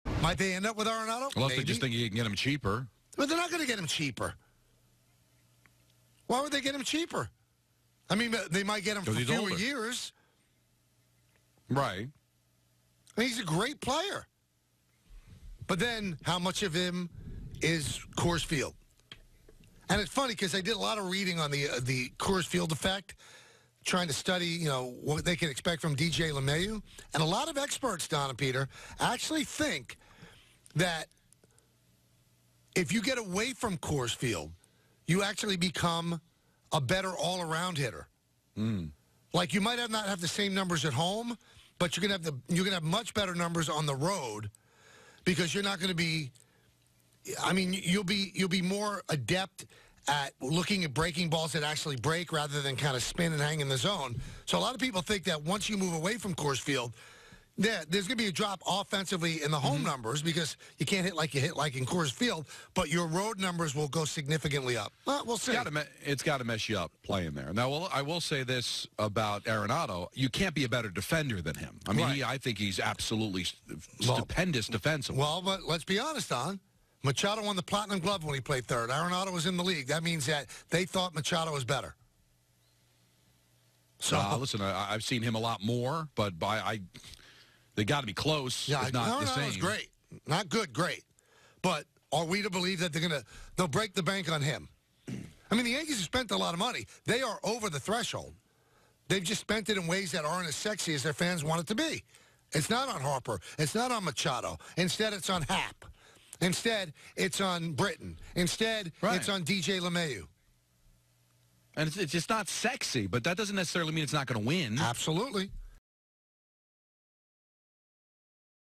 Are the Yankees waiting to sign Nolan Arenado in 2020?
Amid discussions over Manny Machado's future this winter, some suspect the Yankees could be waiting until 2020's free agency class to pounce on Rockies 3B Nolan Arenado.